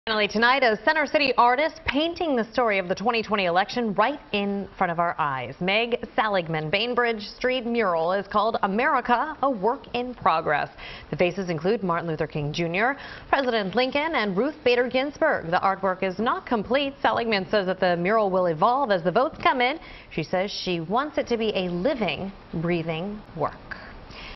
Center City Artist Painting Story Of 2020 Election
Meg Saligman's mural is called "America, A Work In Progress."